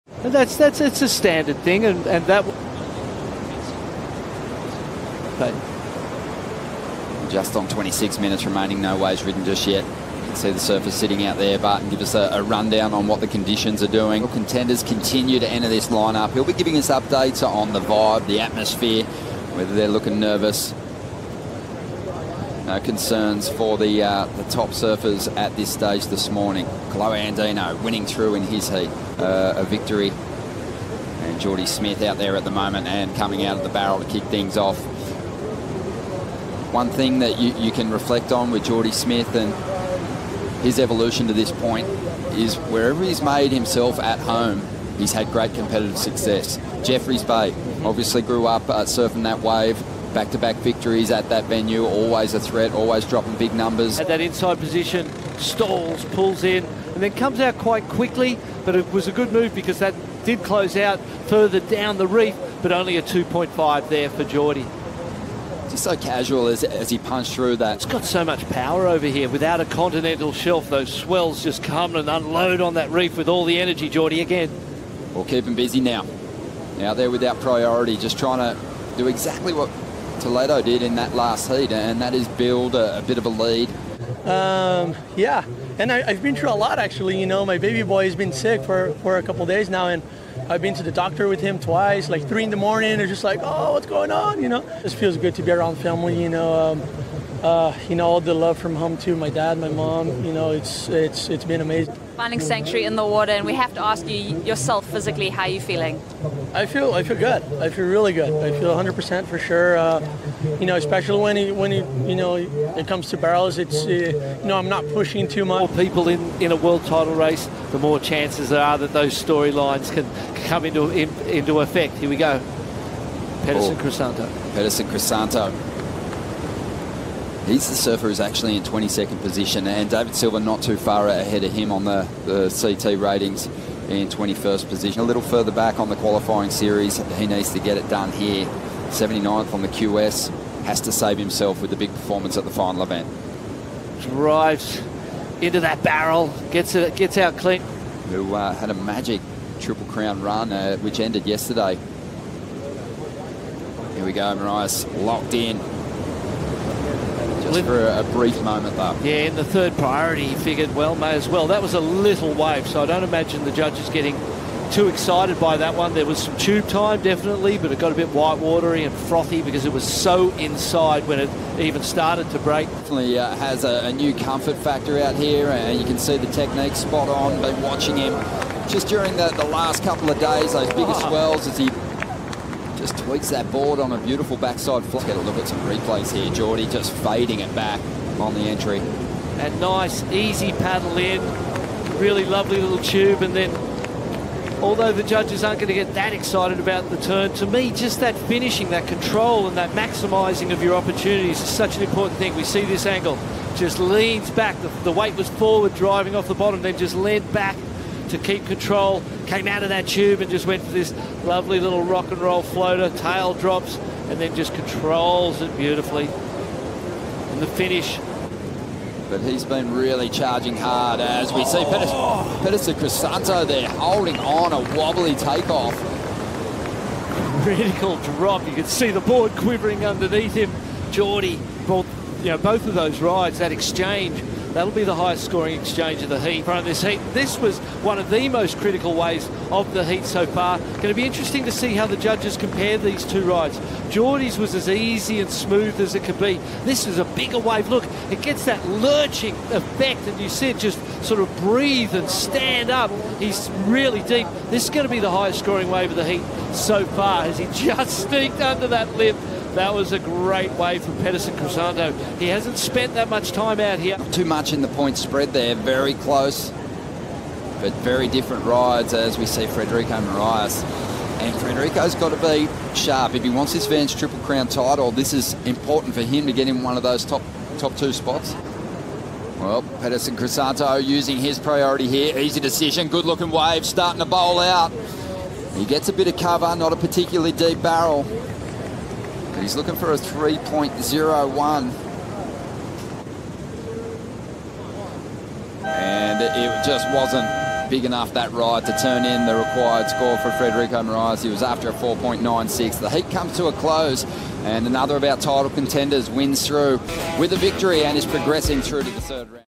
Smith vs. Crisanto vs. Morais - Seeding Round, Heat 4 - Billabong Pipe Masters 2019
Jordy Smith, Peterson Crisanto, and Frederico Morais battle in Seeding Round, Heat 4 at the 2019 Billabong Pipe Masters.